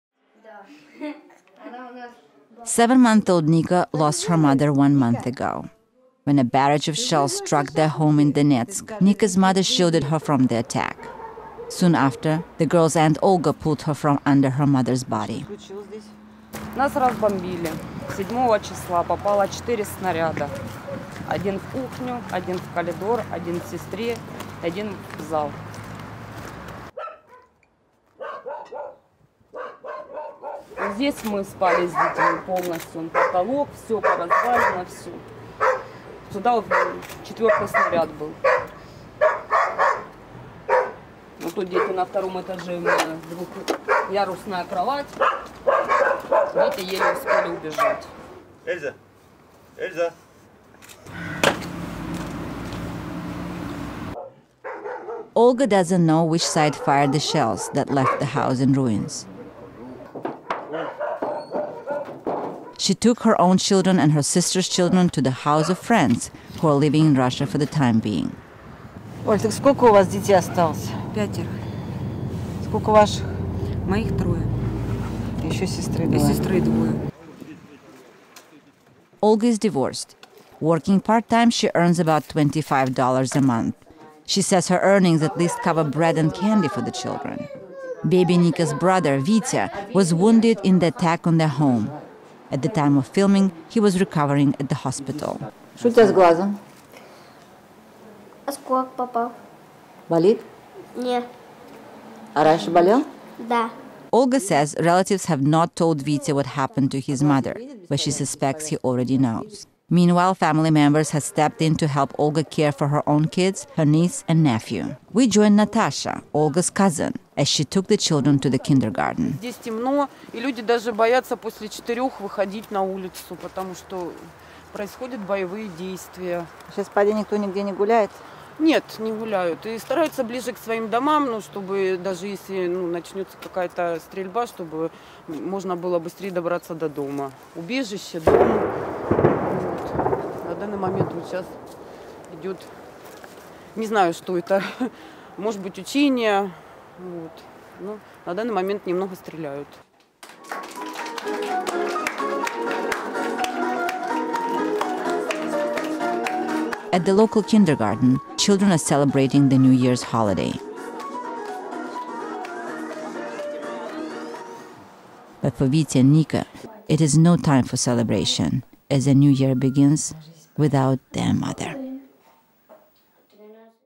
A Family Copes With Tragedy In Wartorn Donetsk
In December, a family in eastern Ukraine was torn apart by fighting between government troops and separatists rebels. Baby Nika and her brother Vitya lost their mother and their home in a shell attack. The children's aunt is now taking care of them and her own young children as they struggle to put their lives back together. (Produced by Shahida Yakub, RFE/RL)